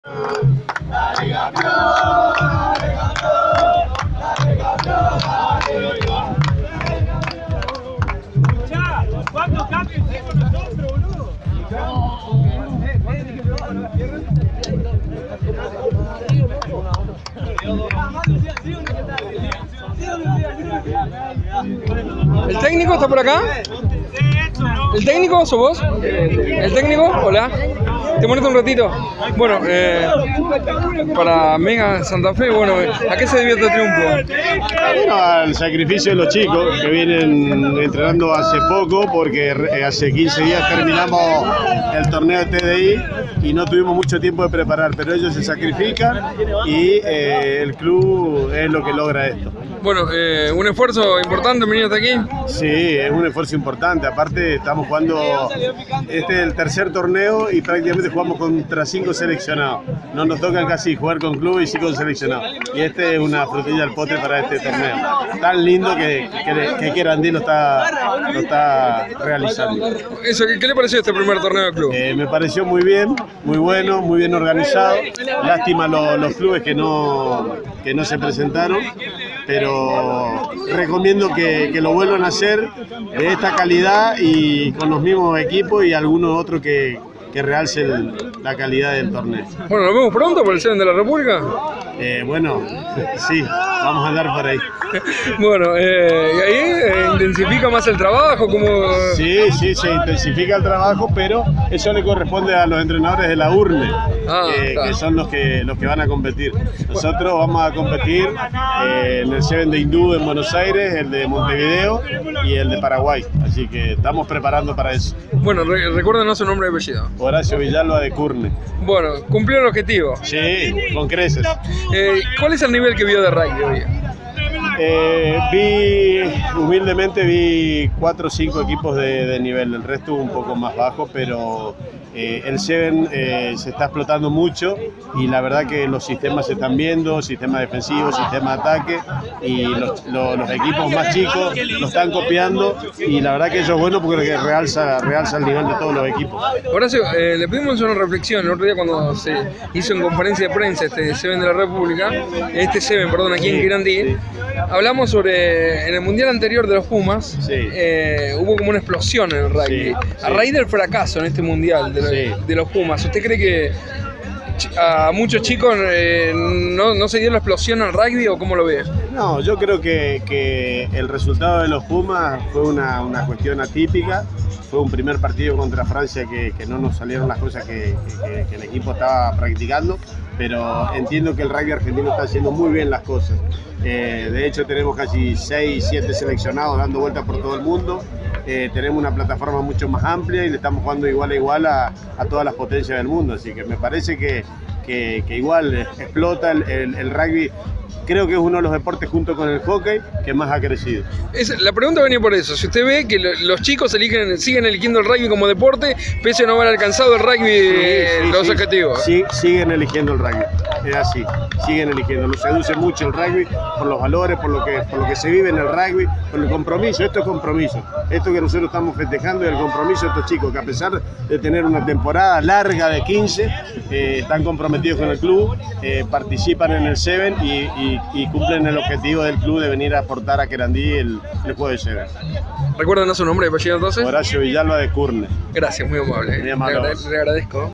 Ganador del torneo club Querandi
El entrenador norteño hablo de dedicación y esfuerzo